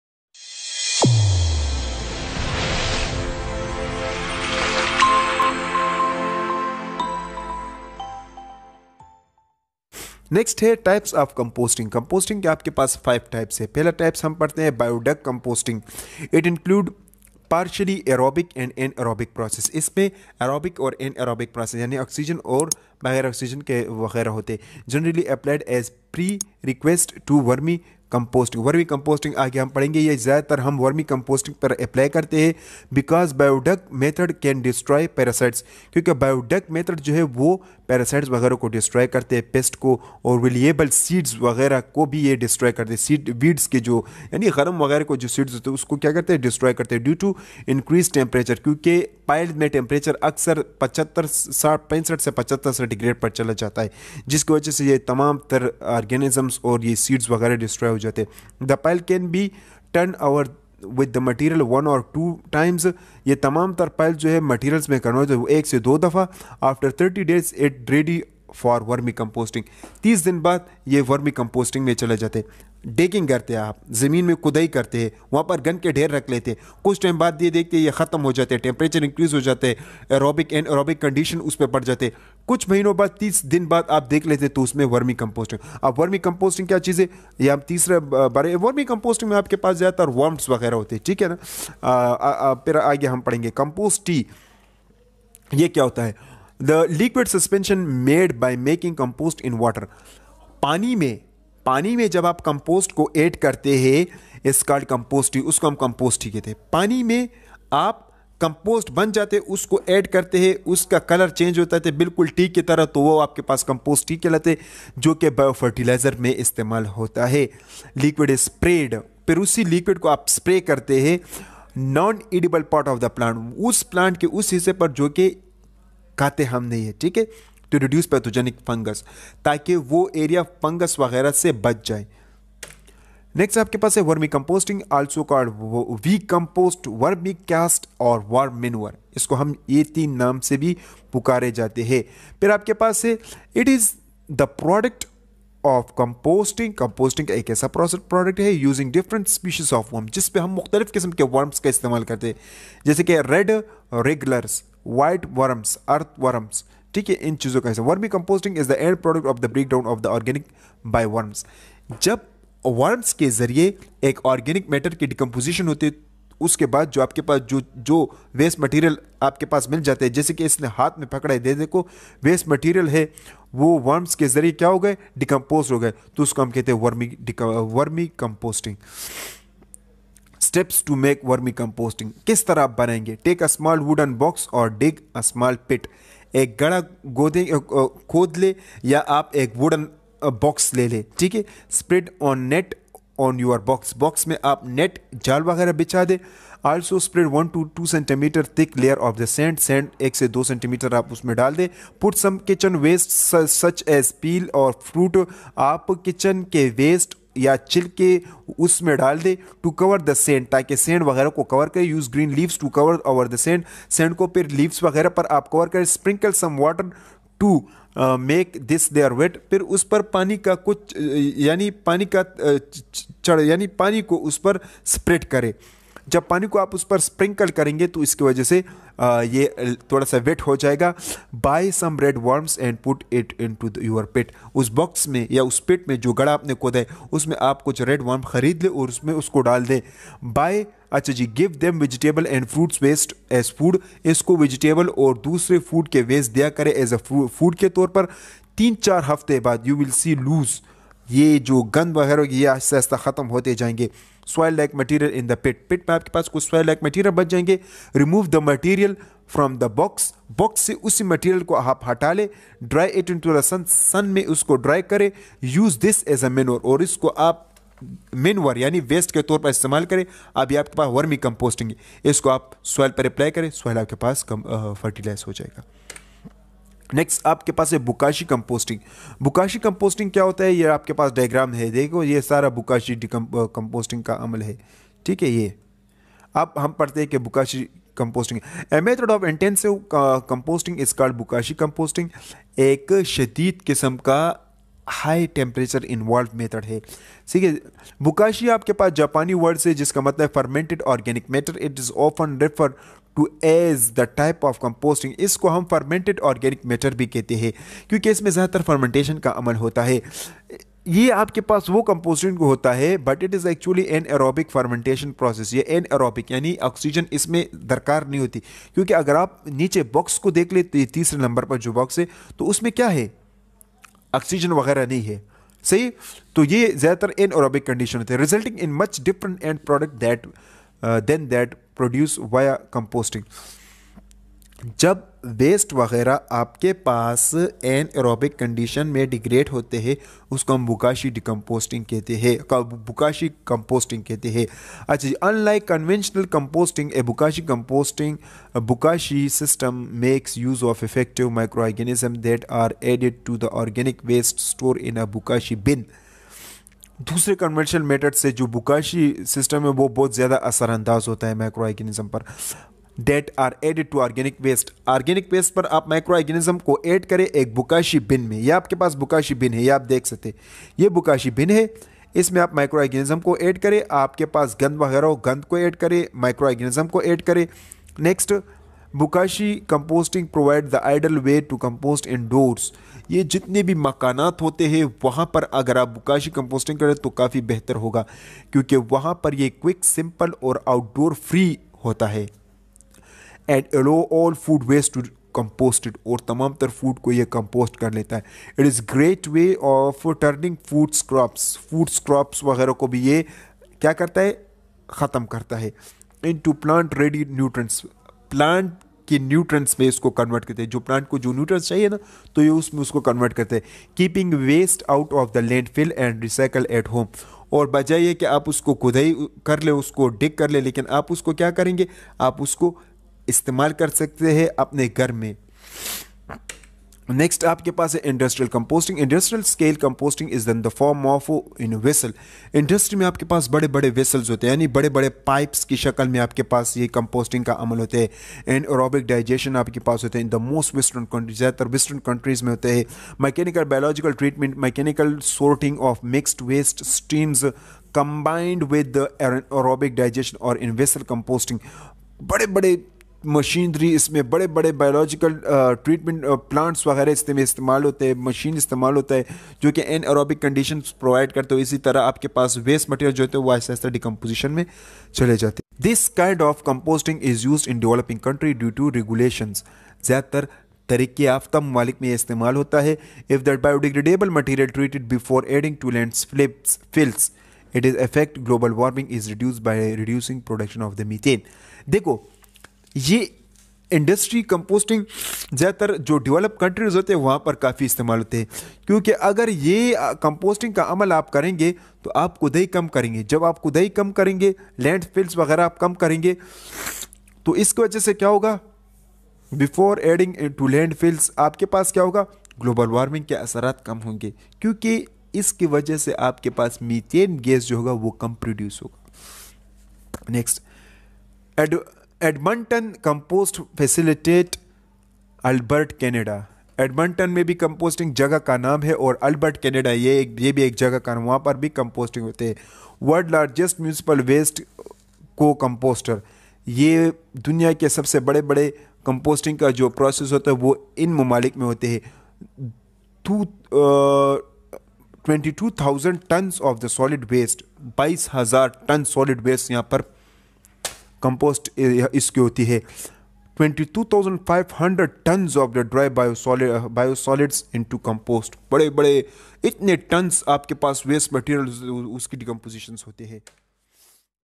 Types of composting
In this video lecture we will explain the types of composting

There are many types of composting but in this video we will explain 5 types of composting

1. Biodung composting

2. Compost tea

3. Vermicomposting

4. Bokashi composting

5. Industrial composting systems